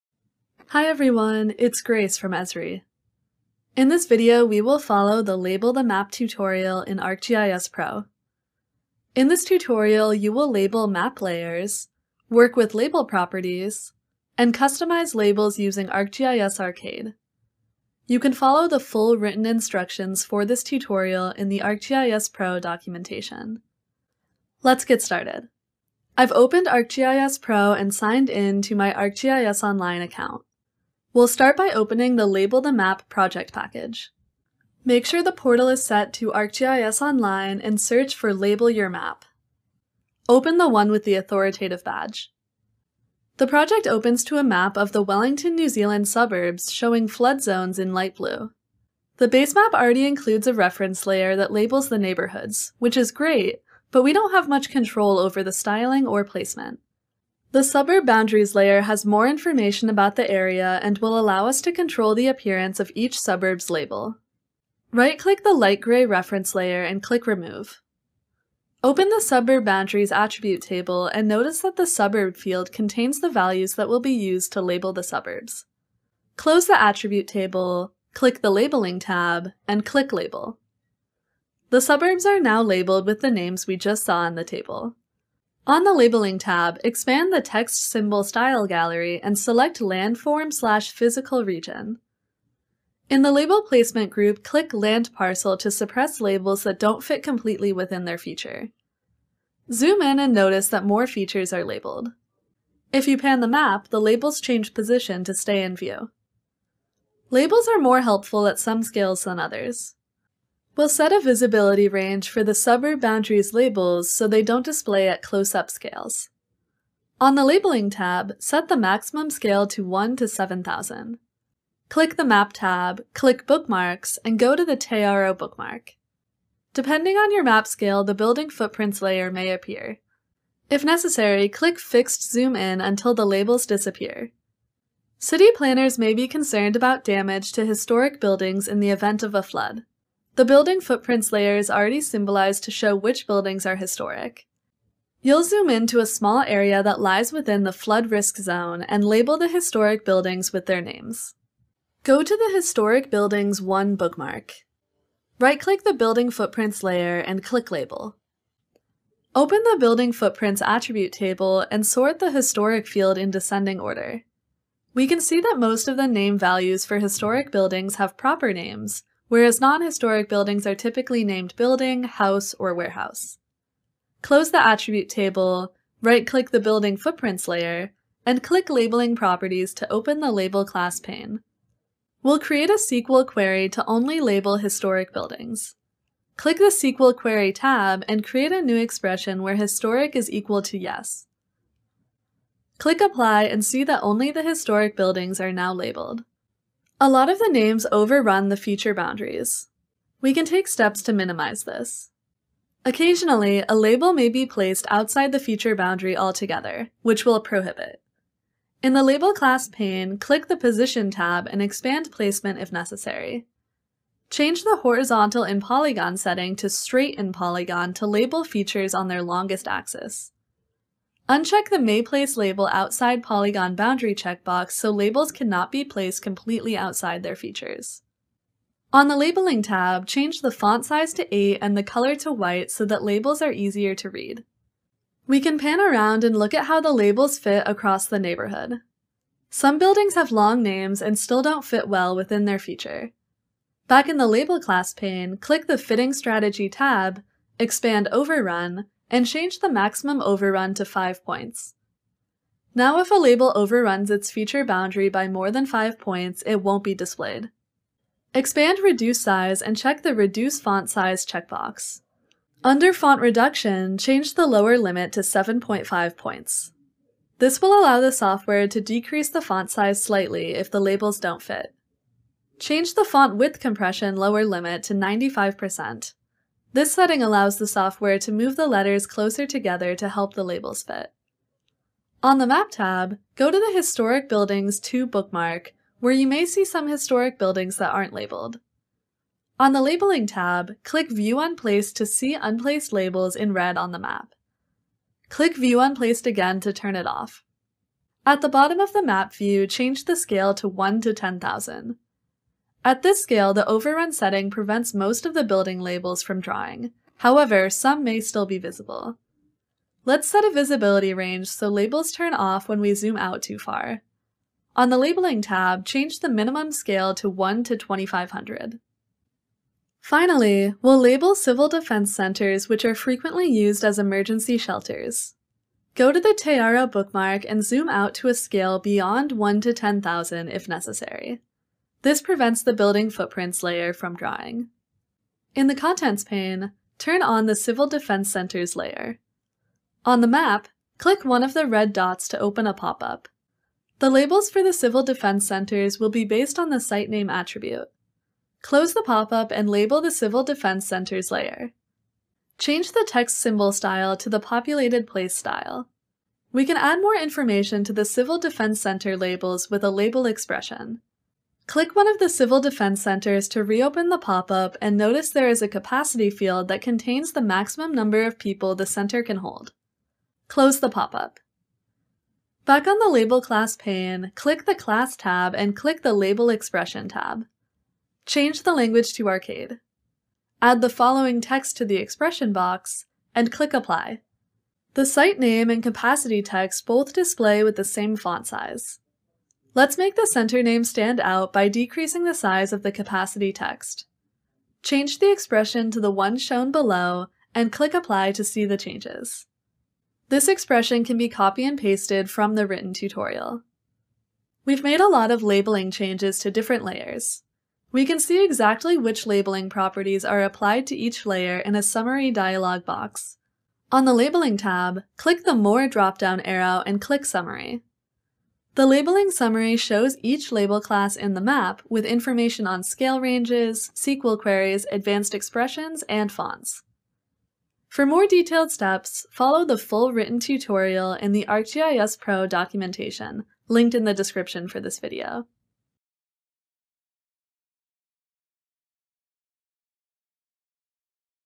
Label the Map in ArcGIS Pro
In this tutorial you will learn how to label map layers, work with label properties, and customize labels using ArcGIS Arcade. Follow the scripted tutorial for the current version of ArcGIS Pro in the ArcGIS Pro documentation.